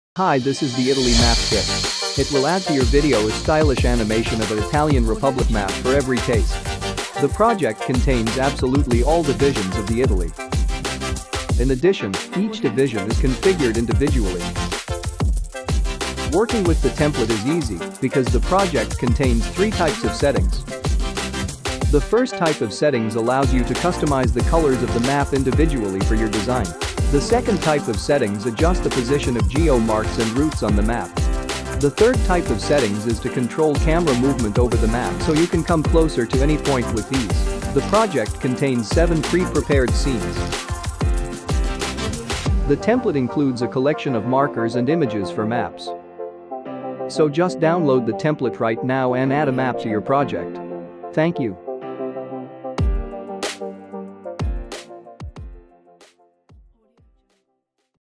Italy Map - Italian Map Kit (After Effects template) | envato animated map
Italy Map – Italian Map Kit — Number One Selling Theme of all navigation presentations and traveling promos

Hi This is the Italy Map Kit. It will add to your video a stylish animation of a Italian Republic map for every taste.

The project contains absolutely all divisions of the Italy.

In addition, each division is configured individually.

Working with the template is easy, because the project contains 3 types of settings.

The first type of settings allows you to customize the colors of the map individually for your design.